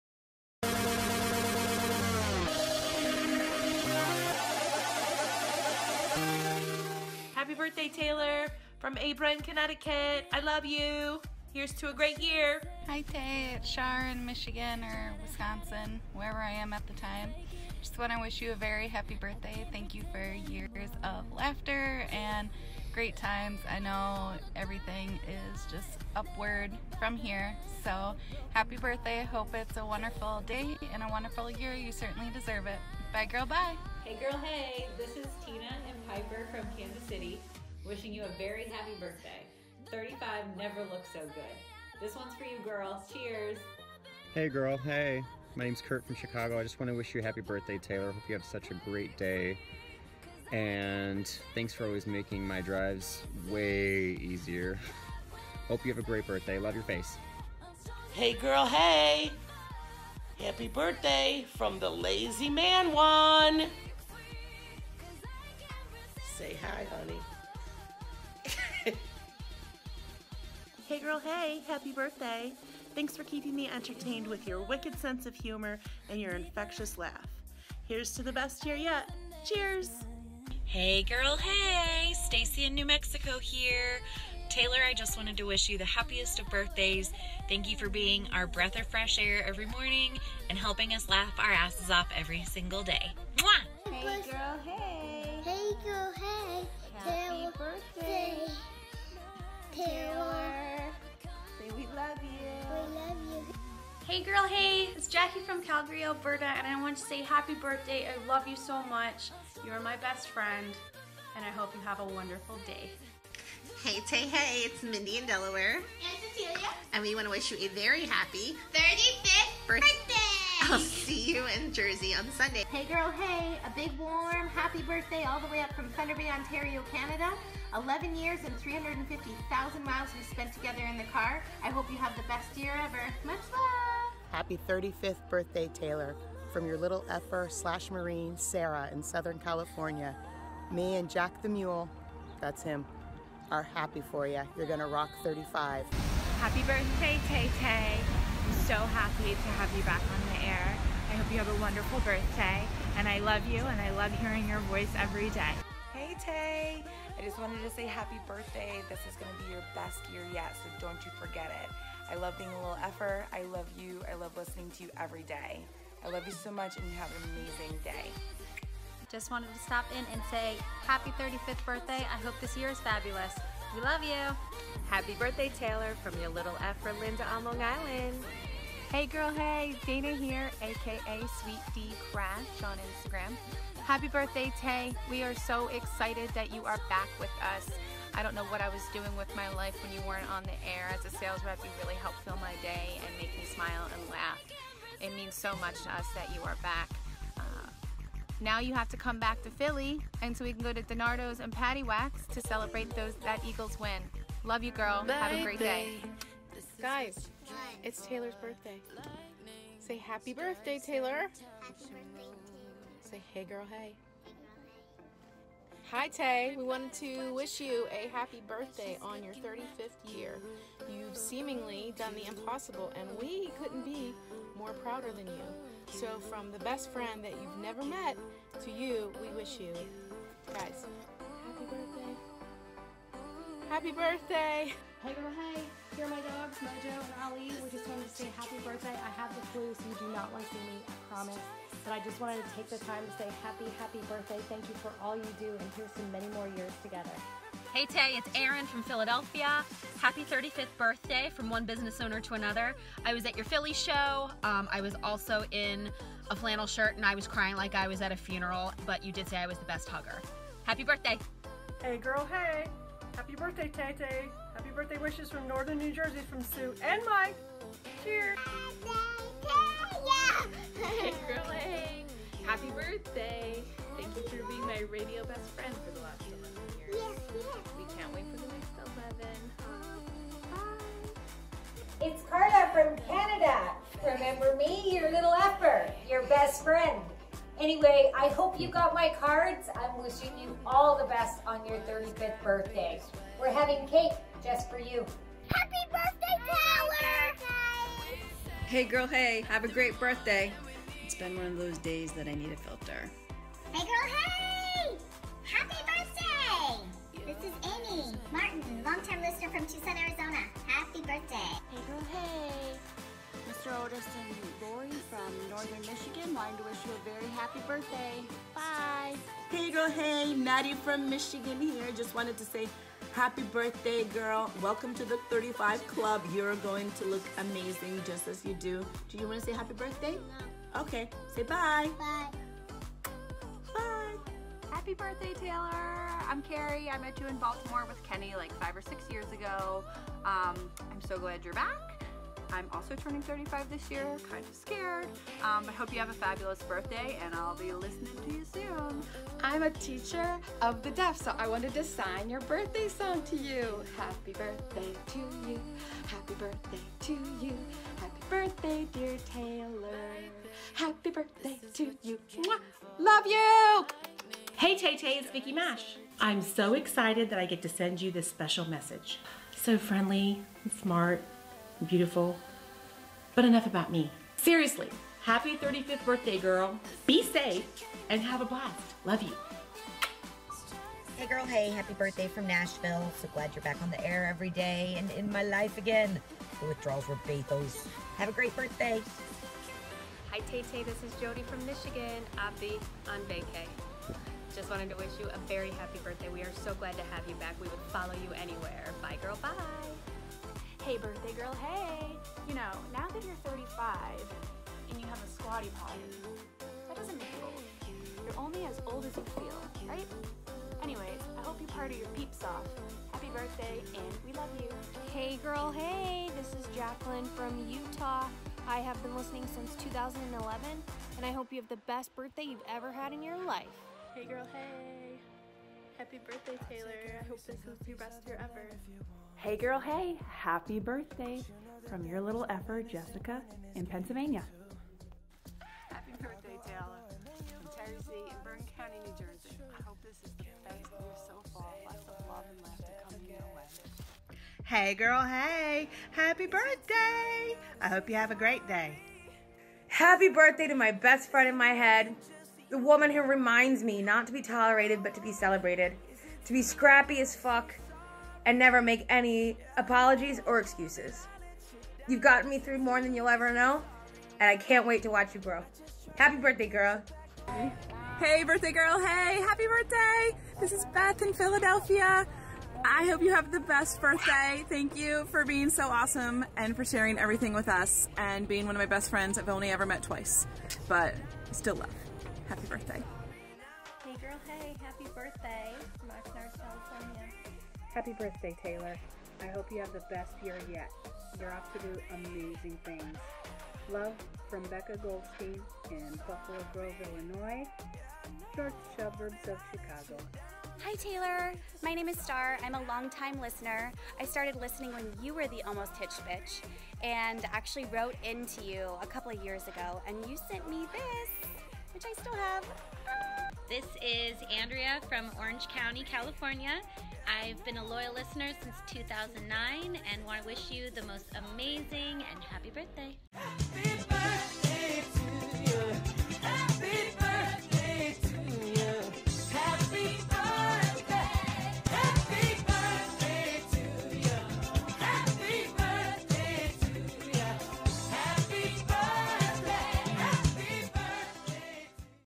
Happy Birthday, Taylor!
Your Lil Effers are so happy to have you back!  We all wish you a very happy birthday!!